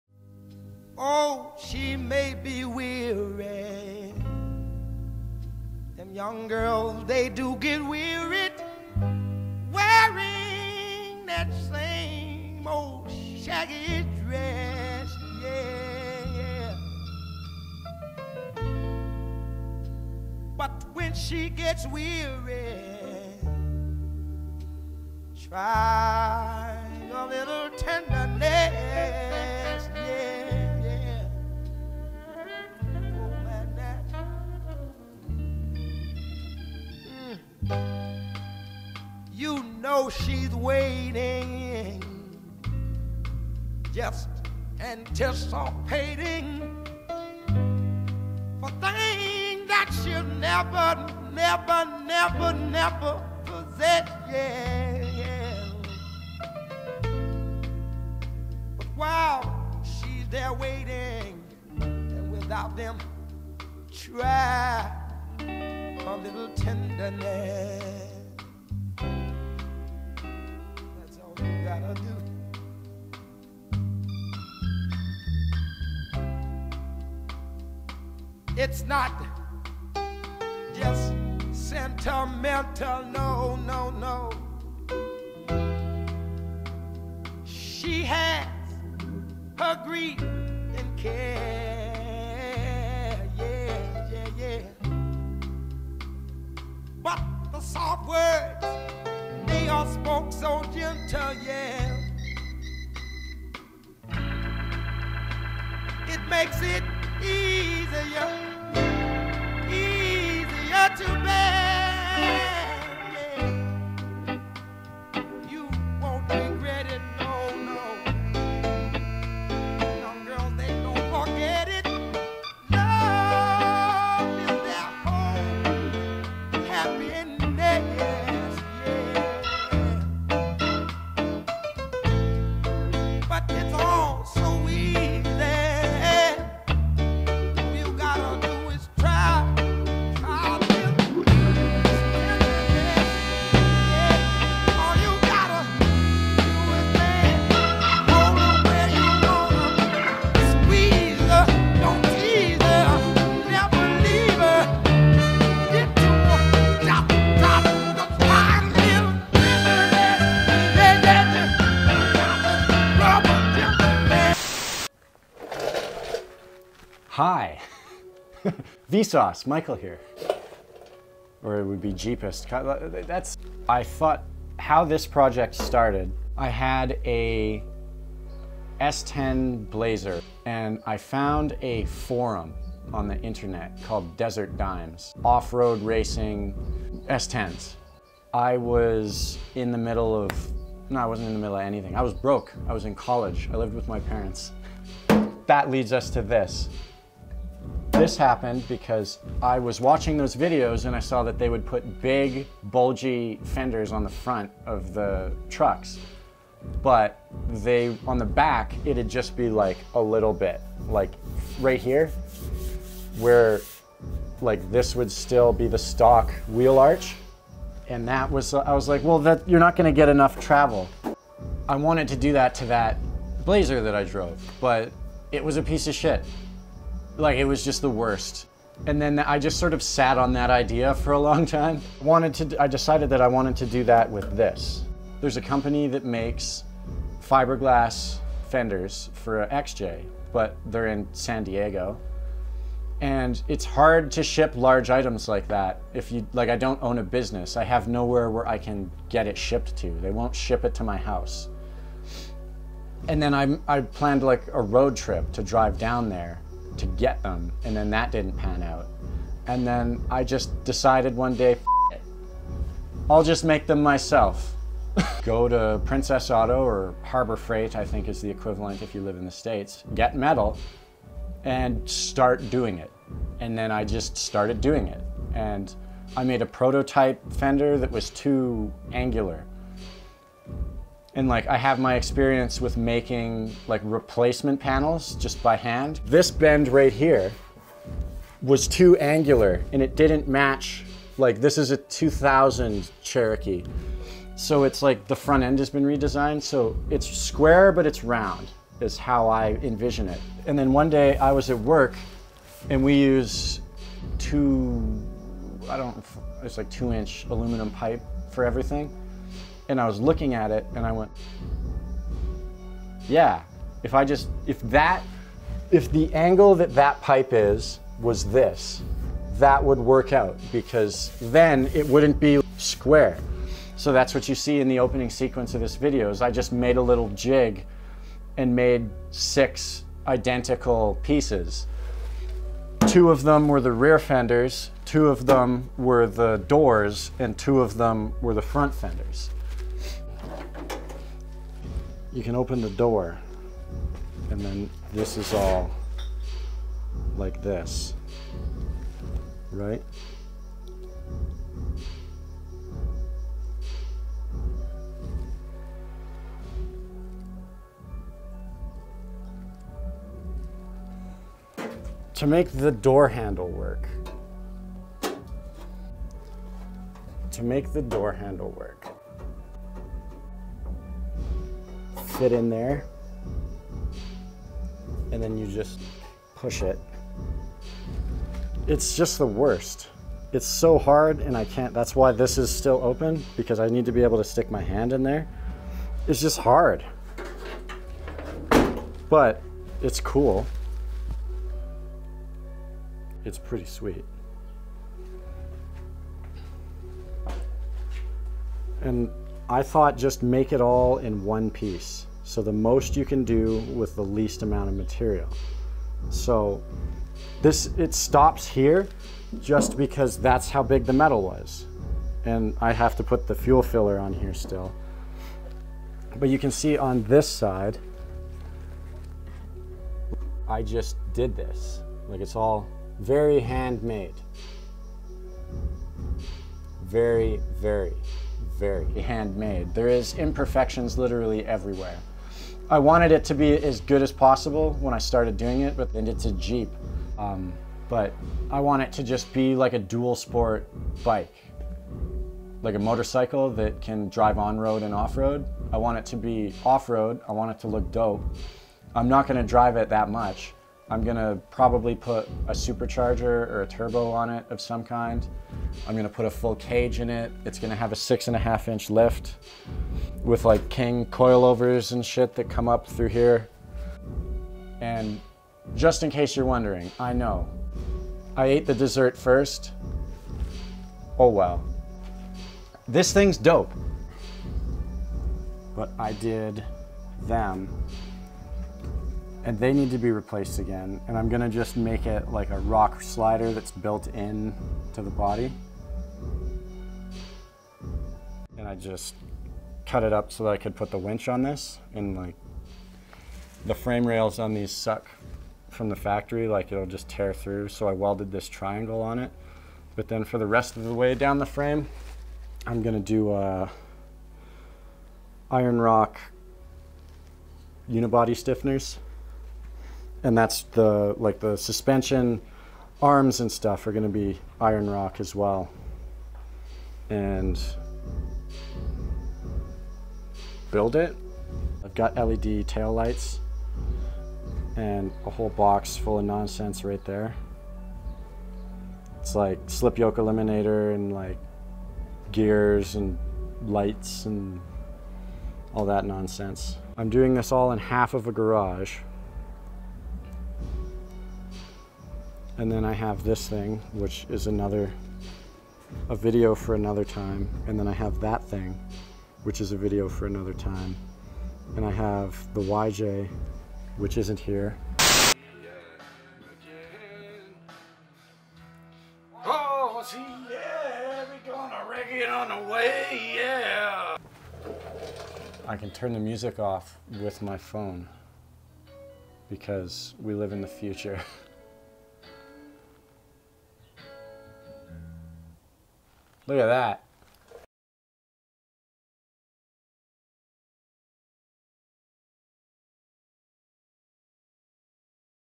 THE JEEPIST EP 10 : 2000 Cherokee Hand Made Wide Body.
2000 Cherokee Im building into a Pre Runner or as pre runner as a broke Canadian can make.

Opening Sequence Shot by:
Tyler "Jinx" Moore
Created By
Kyle Curran